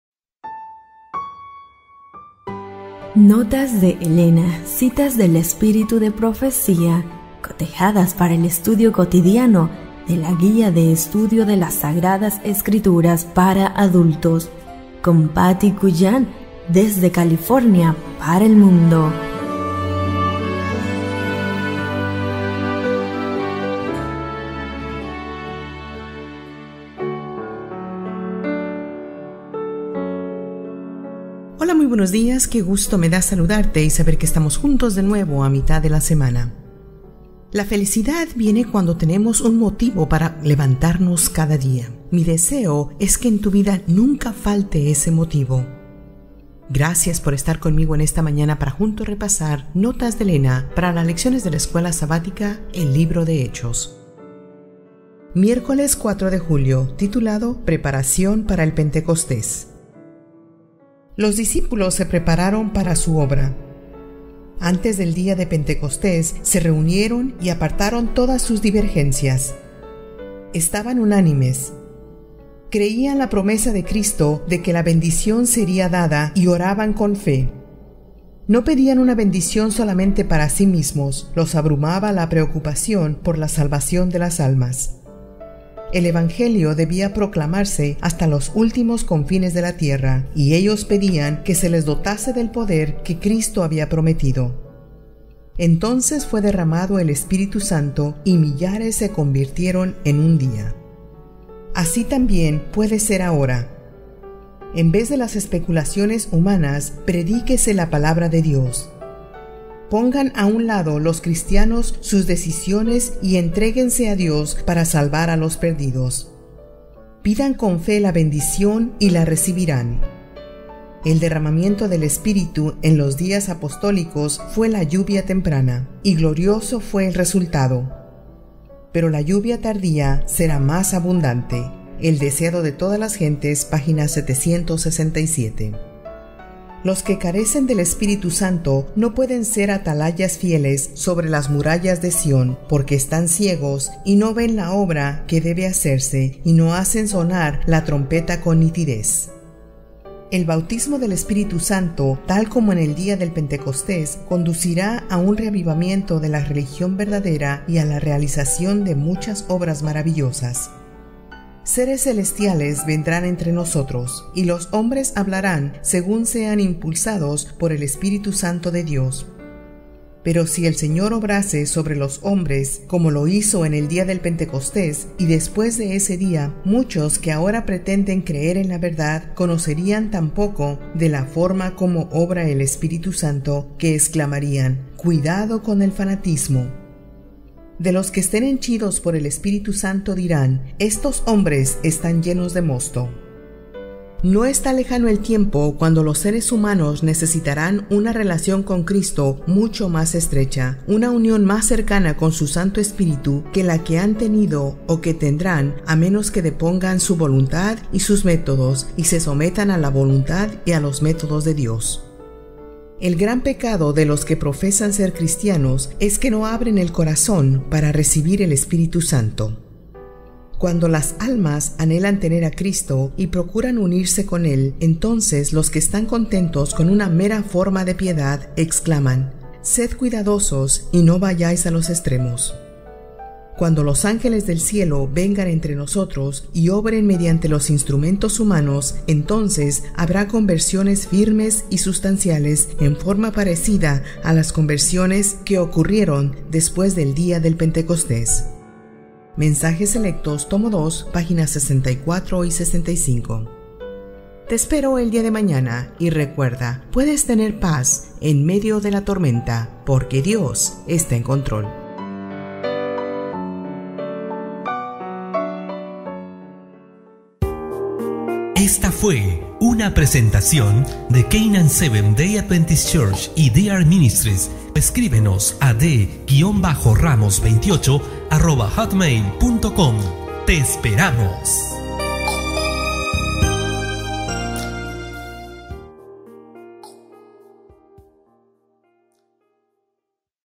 Notas de Elena - 04 de Julio del 2018 - Preparación para el Pentecostés
PREPARACIÓN PARA EL PENTECOSTÉS
Los discípulos se prepararon para su obra. Antes del día de Pentecostés, se reunieron y apartaron todas sus divergencias. Estaban unánimes. Creían la promesa de Cristo de que la bendición sería dada, y oraban con fe. No pedían una bendición solamente para sí mismos; los abrumaba la preocupación por la salvación de las almas. El evangelio debía proclamarse hasta los últimos confines de la tierra, y ellos pedían que se les dotase del poder que Cristo había prometido. Entonces fue derramado el Espíritu Santo, y millares se convirtieron en un día.
Así también puede ser ahora. En vez de las especulaciones humanas, predíquese la Palabra de Dios. Pongan a un lado los cristianos sus disensiones y entréguense a Dios para salvar a los perdidos. Pidan con fe la bendición, y la recibirán. El derramamiento del Espíritu en los días apostólicos fue la "lluvia temprana" [Joel 2:23], y glorioso fue el resultado. Pero la lluvia "tardía" será más abundante (El Deseado de todas las gentes, p. 767).
Los que carecen del Espíritu Santo no pueden ser atalayas fieles sobre las murallas de Sion, porque están ciegos y no ven la obra que debe hacerse, y no hacen sonar la trompeta con nitidez.
El bautismo del Espíritu Santo, tal como en el día de Pentecostés, conducirá a un reavivamiento de la religión verdadera y a la realización de muchas obras maravillosas. Seres celestiales vendrán entre nosotros, y los hombres hablarán según sean impulsados por el Espíritu Santo de Dios. Pero si el Señor obrase sobre los hombres como lo hizo en el día de Pentecostés y después de ese día, muchos que ahora pretenden creer en la verdad conocerían tan poco de la forma como obra el Espíritu Santo, que exclamarían: "¡Cuidado con el fanatismo!" De los que estén henchidos por el Espíritu Santo dirían: "Estos hombres están llenos de mosto".
No está lejano el tiempo cuando los seres humanos necesitarán una relación con Cristo mucho más estrecha, una unión mucho más cercana con su Santo Espíritu, que la que han tenido o que tendrán, a menos que depongan su voluntad y sus métodos, y se sometan a la voluntad y los métodos de Dios. El gran pecado de los que profesan ser cristianos es que no abren el corazón para recibir el Espíritu Santo. Cuando las almas anhelan tener a Cristo, y procuran unirse con él, entonces los que están contentos con una mera forma de piedad, exclaman: "Sed cuidadosos y no vayáis a los extremos". Cuando los ángeles del cielo vengan entre nosotros, y obren mediante los instrumentos humanos, entonces habrá conversiones firmes y sustanciales, en forma parecida a las conversiones que ocurrieron después del día de Pentecostés (Mensajes selectos, t. 2, pp. 64, 65).